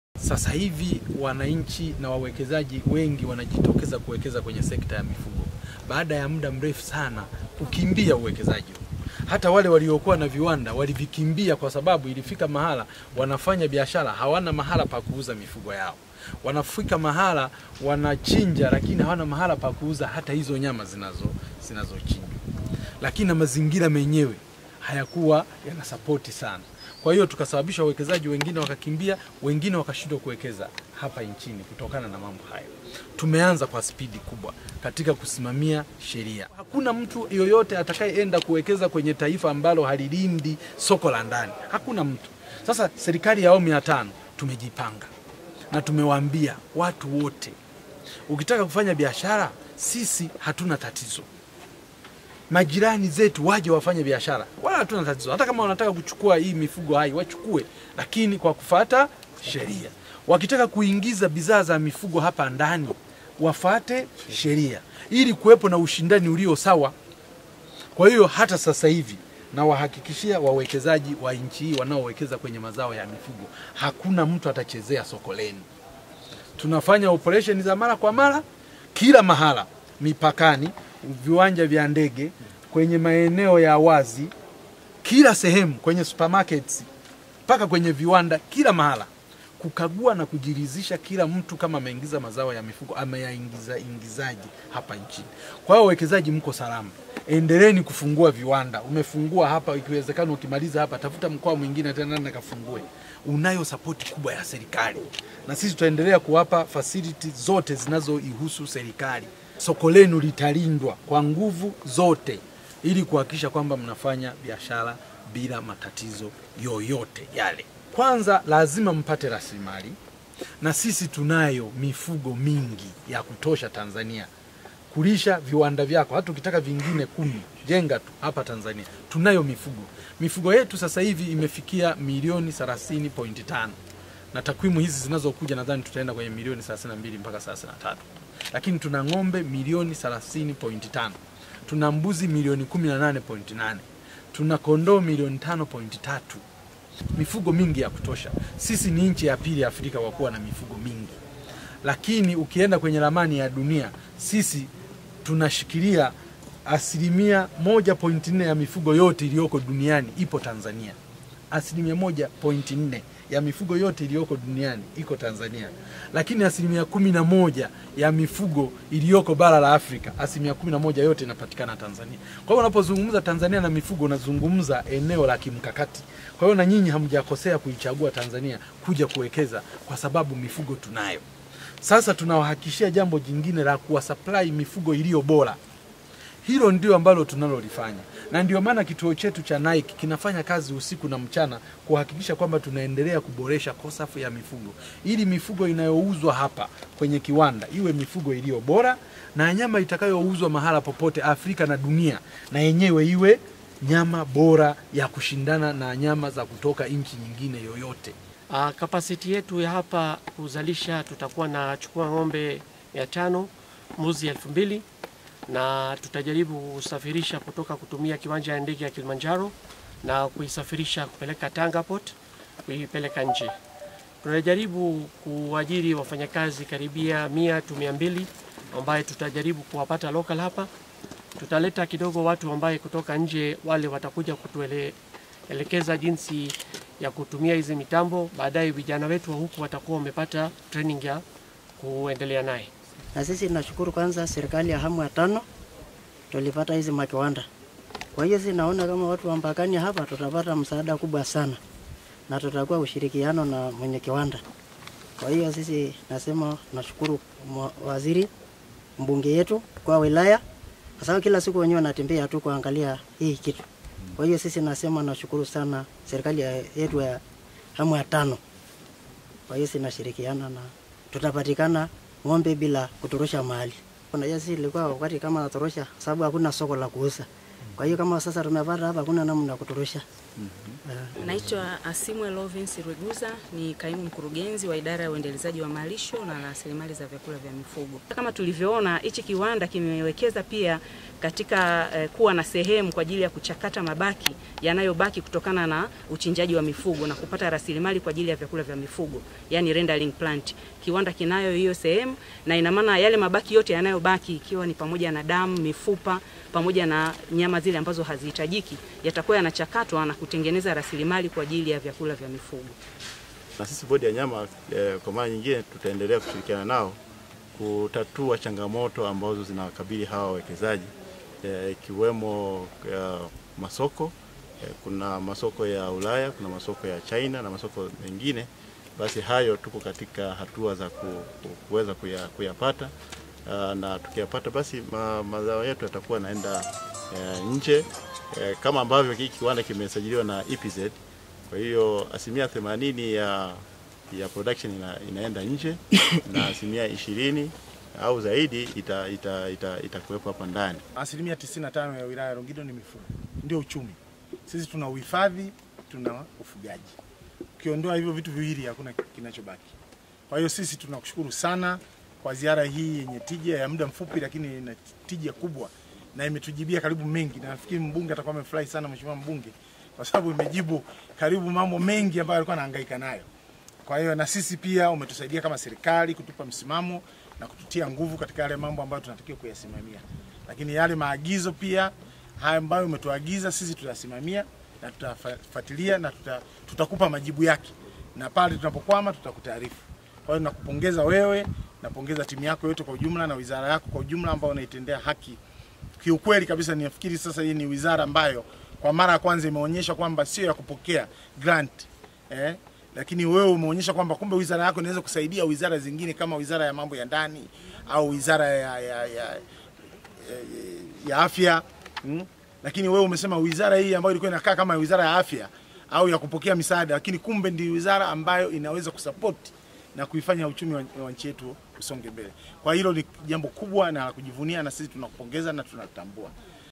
Waziri Mpina: Hakuna atakayetuchezea | Tumeanza kwa spidi kubwa
Waziri wa Mifugo na Uvuvi, Luhaga Mpina amewahakikishia wawekezaji wa viwanda vya kuchakata mazao mazao ya mifugo nchini kuwa hakuna mtu yeyote atakayechezea soko la bidhaa hizo kwani Serikali iko macho wakati wote kudhibiti uingizaji holela wa mazao ya mifugo kutoka nje ya nchi.

Hivyo amesititiza kuwa hatua zinazochukuliwa na Serikali kudhibiti utoroshaji wa mifugo nje ya nchi na kudhibiti uingizaji holela wa bidhaa za nyama na maziwa hazikulenga kumuonea mtu bali kutengeneza mazingira mazuri kwa wawekezaji wa viwanda hivyo kuwekeza nchini.

Mpina alisema kuanza kwa viwanda hivyo vipya ni uthibitisho kuwa Serikali imeweka mazingira wezeshi kwa wawekezaji kuja kuwekeza nchini baada ya wawekezaji wengi siku za nyuma kulazimika kufunga viwanda vyao na kukimbia kutokana na utitiri wa bidhaa zilizoingia nchini kwa njia za panya na kutokuwepo ushindani ulio sawa kwenye biashara.